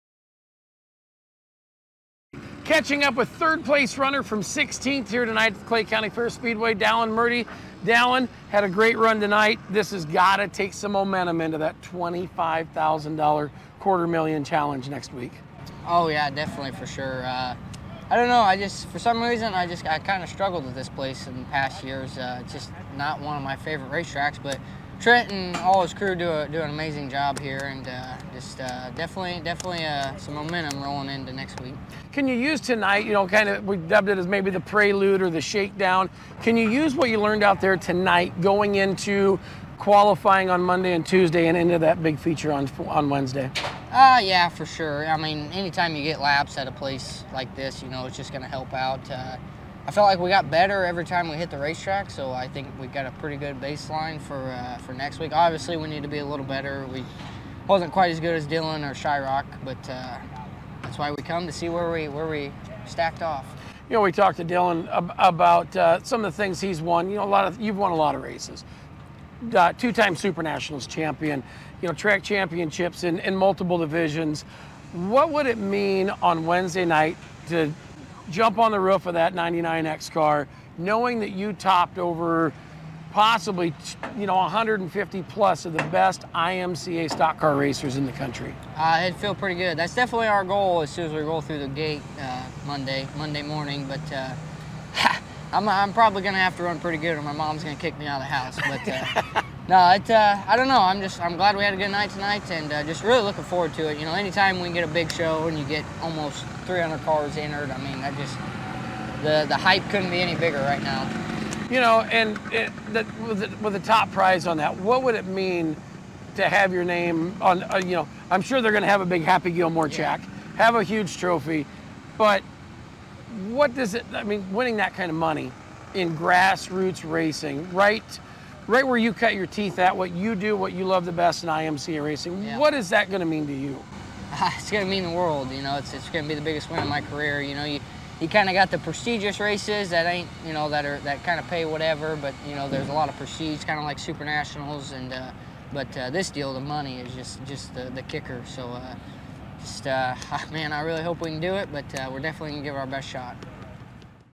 Dallon Murty | Post Race Chat | Clay County Fair Speedway | 5-14-2024
Dallon Murty shares his thoughts on the upcoming $250,000 Challenge after wheeling his IMCA Stock Car around the Clay County Fair Speedway against 46 other competitors.